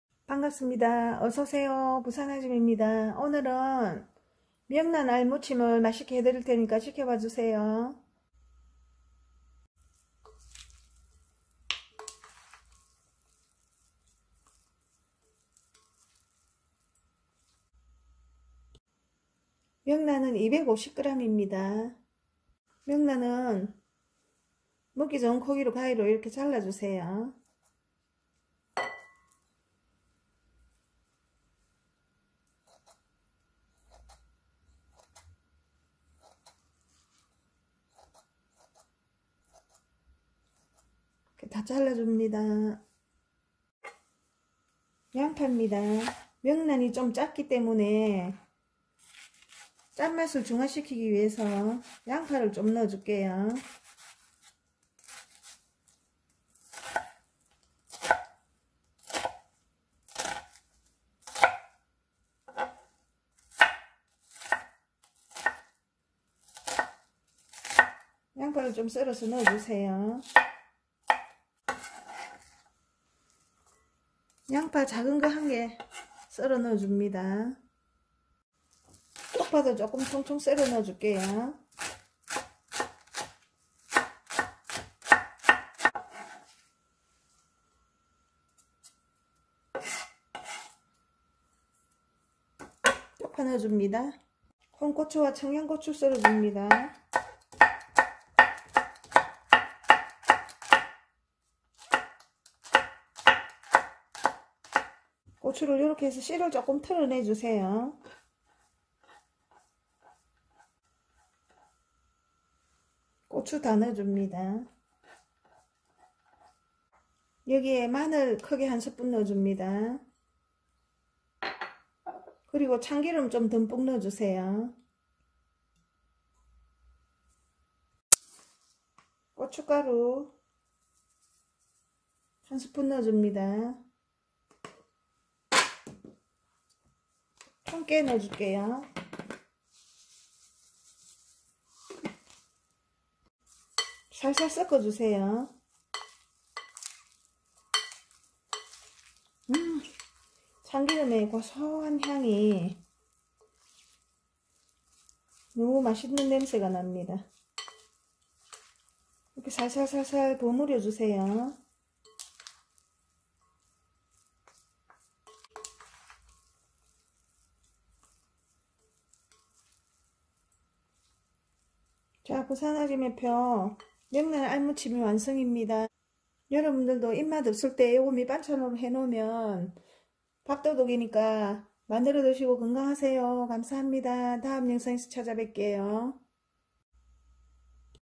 명란알무침~입맛 저격수 밥도둑~톡톡 터지는 알들의 반란~짭조름한맛 잡아주는 양파와 청양고추의 맵삭함이 맛을 업시킴~설명란에 명절음식영상모음 참고하세요(부산아지매레시피)
명절음식영상모음 아래영문을 클릭하세요

밥도둑~명란알젓 양념무침
주-명란250G
부-참기름,마늘,쪽파,양파,고추가루,통깨,청양홍고추,홍고추